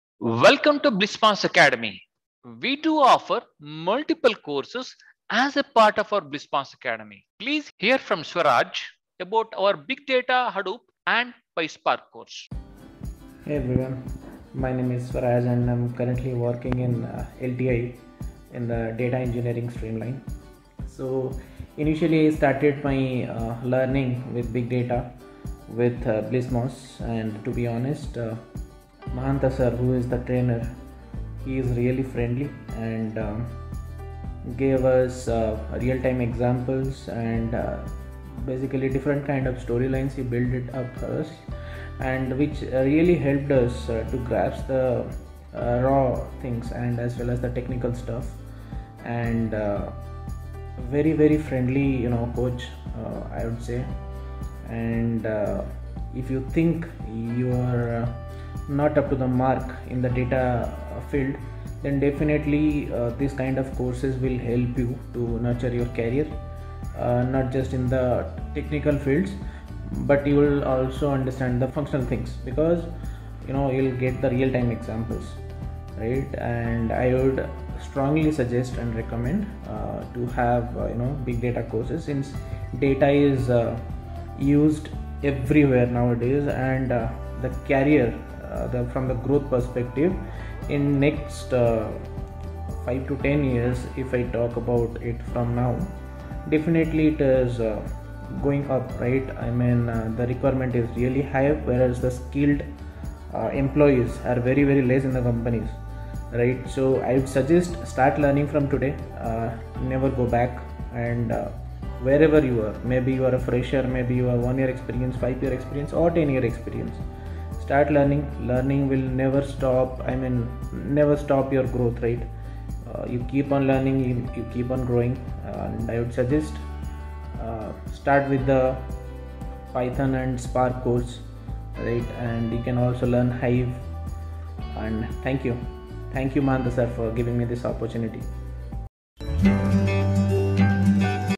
Success Stories | Transformation from Technical Support to Big Data Engineer
Blismos Academy has a track record of enabling and empowering people with the required knowledge for career transformation.
Don't just believe us. Check out some wonderful things said by one of our participants Swaraj and his experience
about how our training helped him to stay updated with evolving technologies for his success.